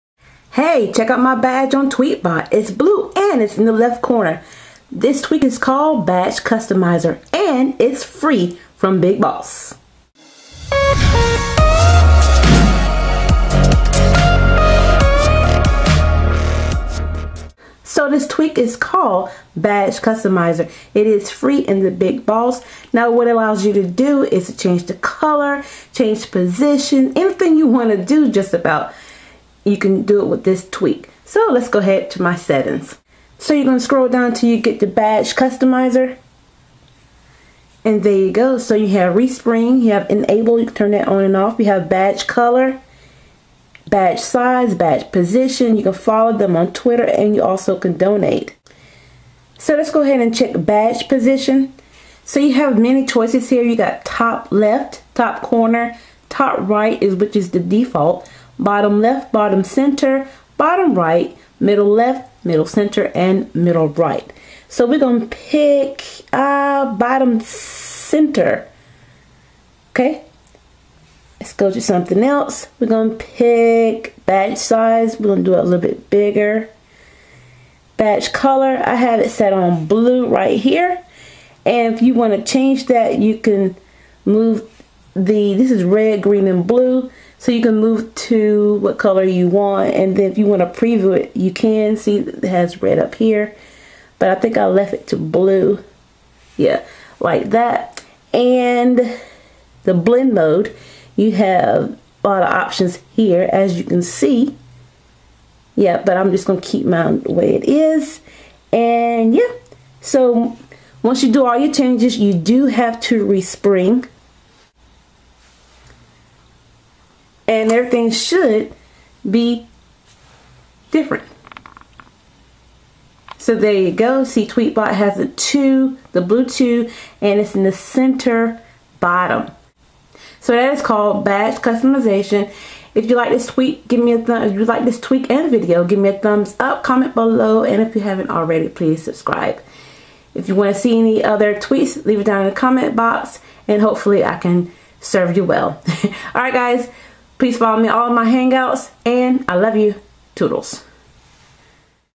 Badge Customizer (Cydia Tweak)
In this video I show you a Cydia tweak call Badge Customizer for iOS 6!

Name: Badge Customizer
Repo: BigBoss
Price: Free
Author:Merdok

Theme used in this video: Anbu